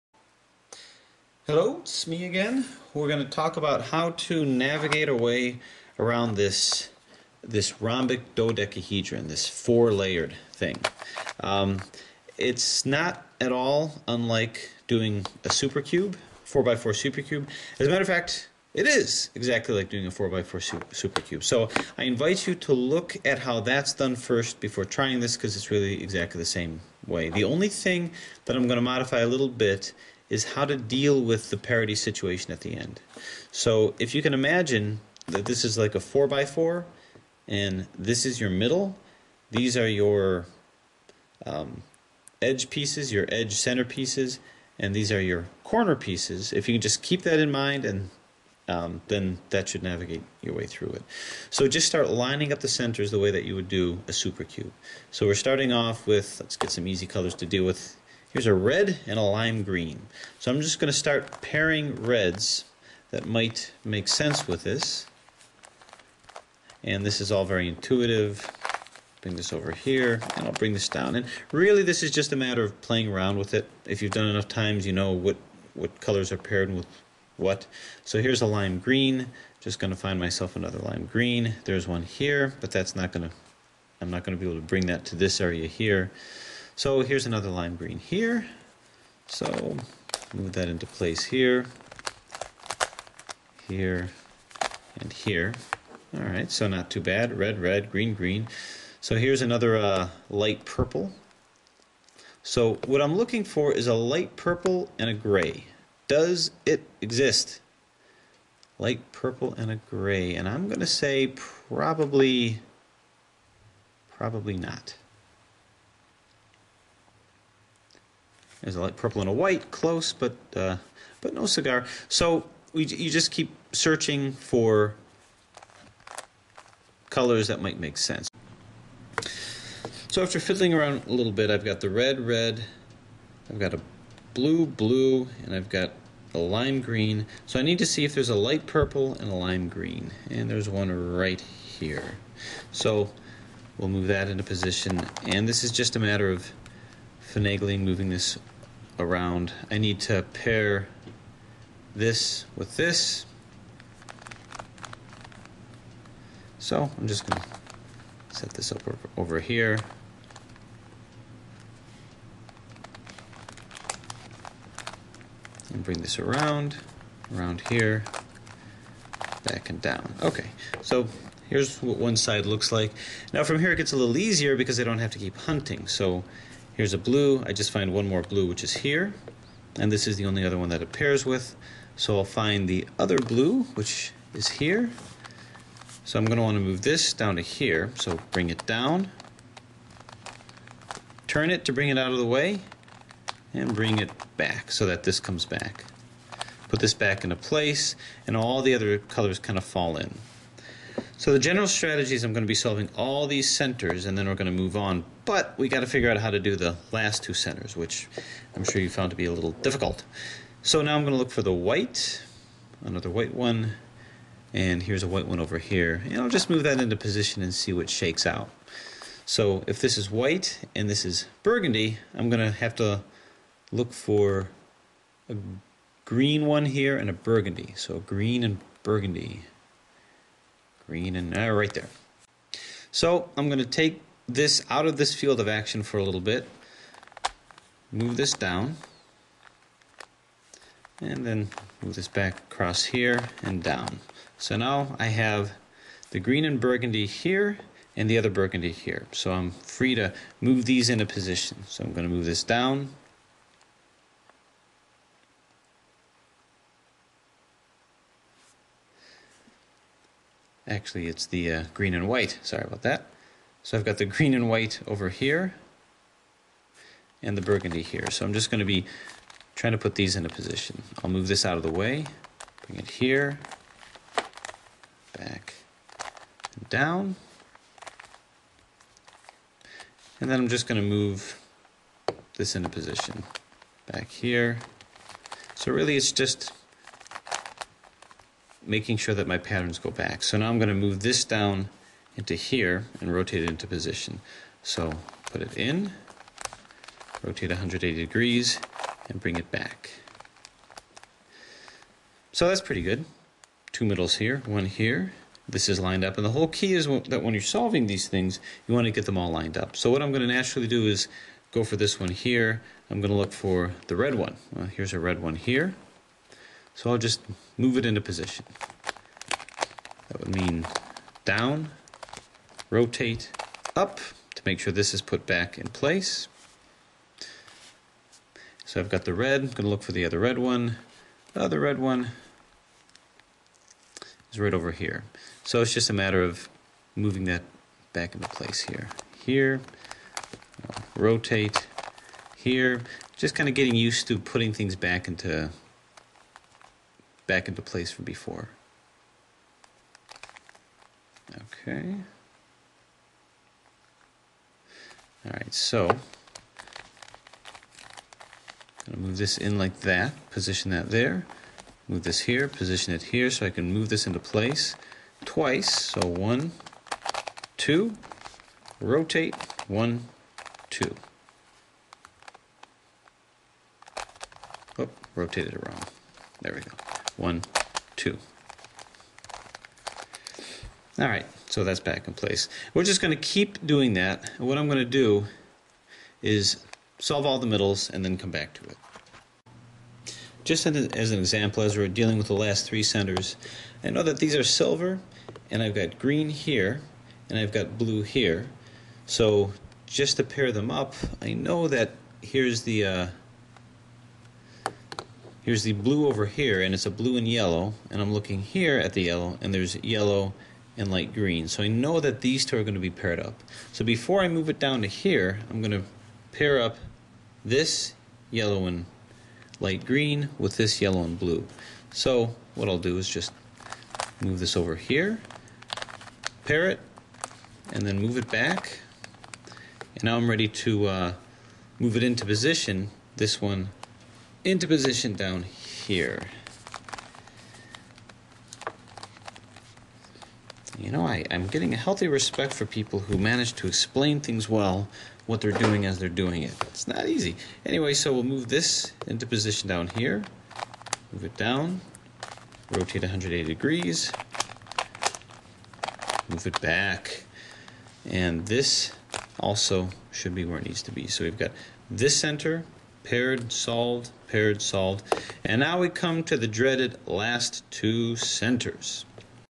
Lanlan 4 layer rhombic dodecahedron tutorial part 1
How to solve a 4 layer rhombic dodecahedron.  Lining up all but the last 2 centers.  Really just strategy, no algorithms.  Go to part 2 for all the exciting parts if stuck at the last 2 centers!